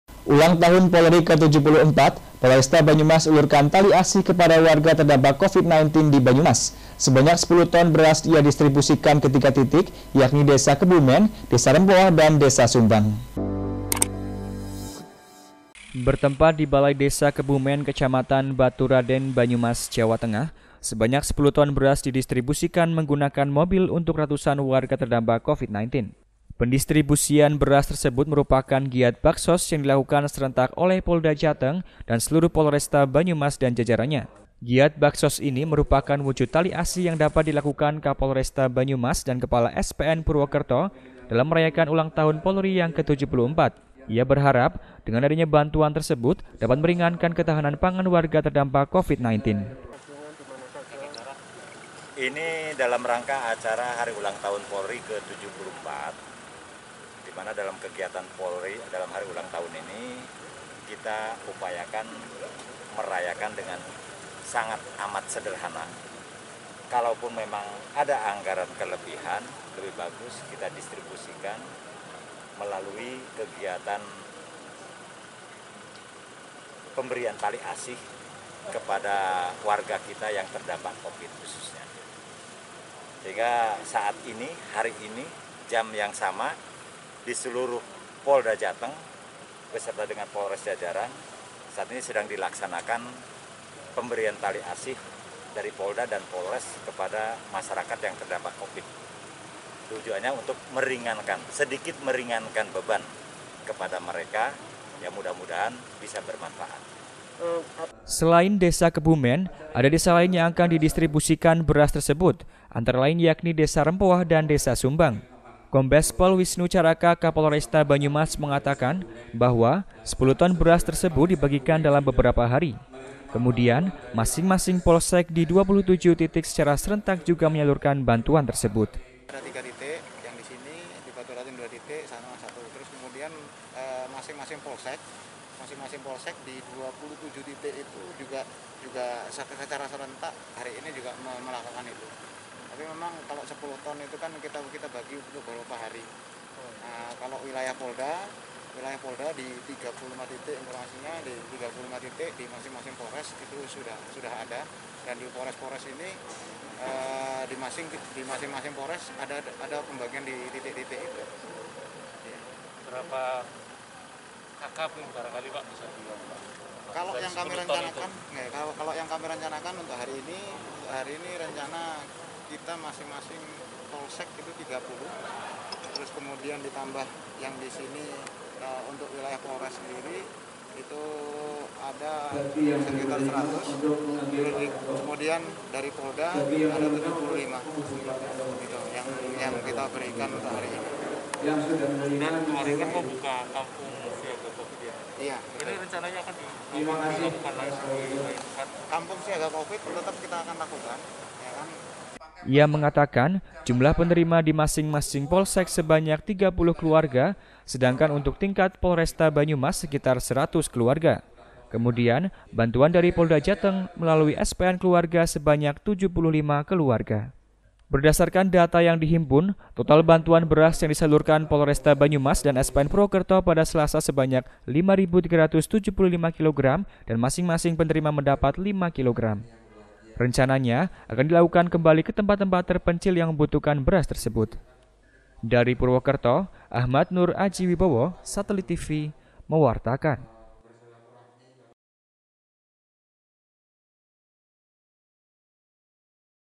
ULANG TAHUN POLRI KE 74, POLRESTA BANYUMAS ULURKAN TALI ASIH KEPADA WARGA TERDAMPAK COVID 19
Ulang tahun Polri ke 74, Polresta Banyumas ulurkan tali asih kepada warga terdampak Covid 19 di Banyumas. Sebanyak 10 Ton beras ia distribusikan ke tiga titik, yakni Desa Kebumen, Desa Rempoah, dan Desa Sumbang.